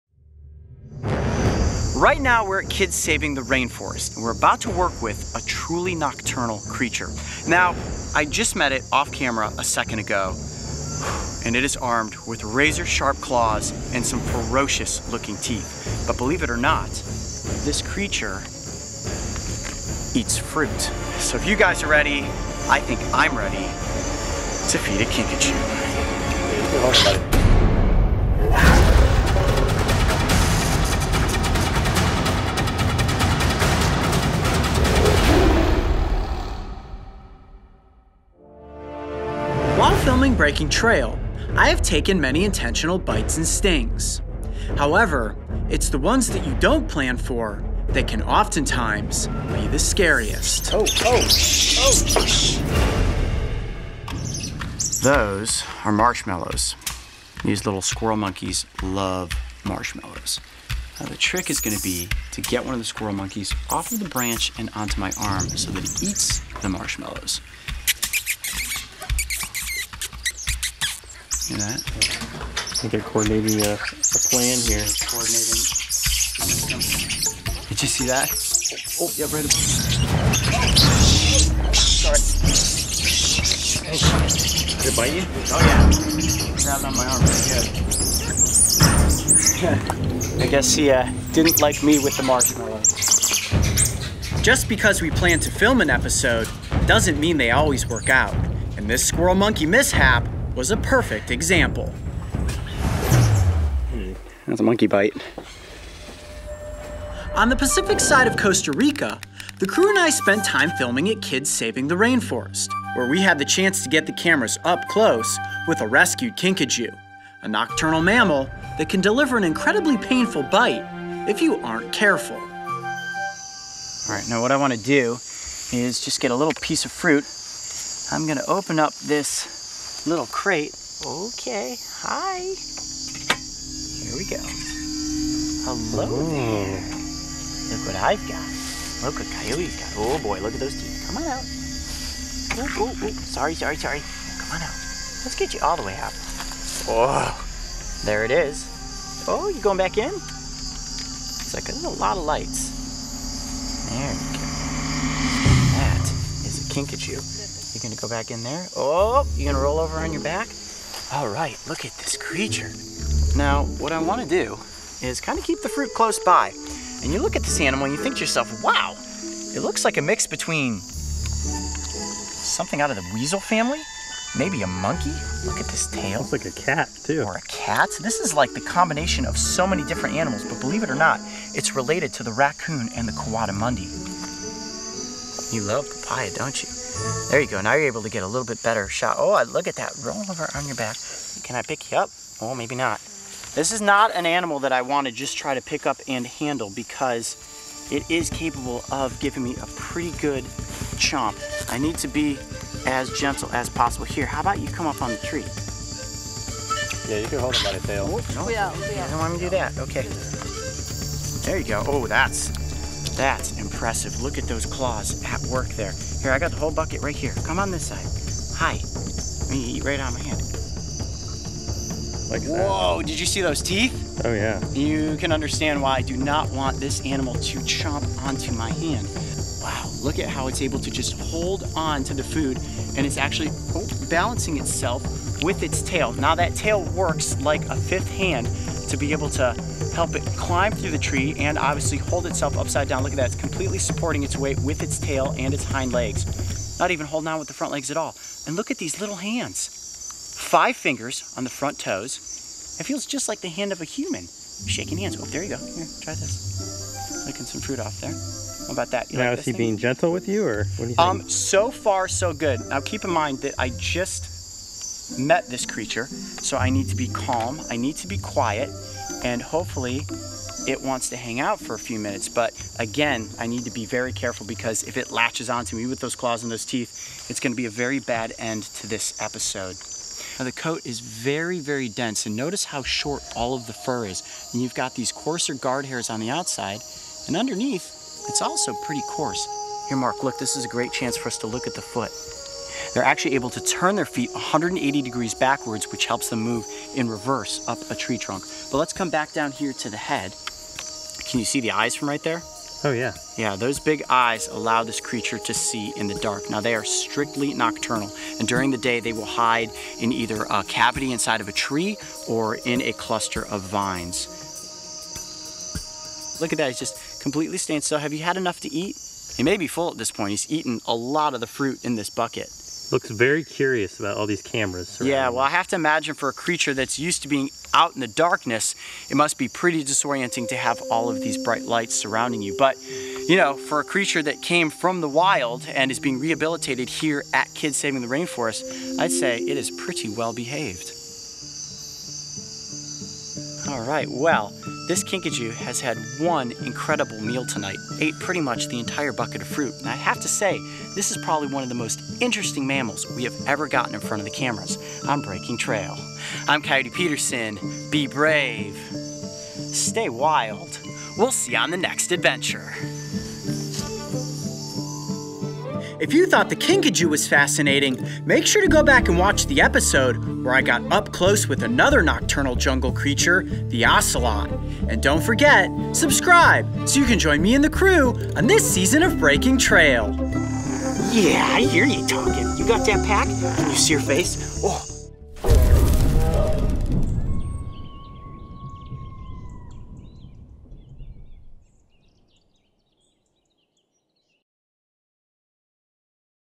SUPER CUTE Kinkajou!
On this episode of Breaking Trail, Coyote is attacked by monkeys and  encounters one of the most mysterious creatures in the rainforest, the Kinkajou!

A Kinka-what? While these nocturnal animals are undoubtably adorable what exactly are they? Well it might surprise you to find out they are not related to monkeys, cats or weasels but are actually a relative of the raccoon!

Also related to Coatimundis, these tree dwelling mammals can often be heard calling in the nighttime canopies of the rainforest in Central America.

Get ready to meet the super cute Kinkajou!

Big thanks to everyone at Kids Saving the Rainforest for hosting the crew at this location. To find out more about their organization please visit their

*Kids Saving the Rainforest never lets the public touch or see the baby wildlife.  We only do so when the filming is to educate, which is our mission.  All wildlife caretakers were present during filming to guarantee the animal's well-being and assure to that they weren't under stress. Remember,  it is never acceptable to have a wild animal as a pet.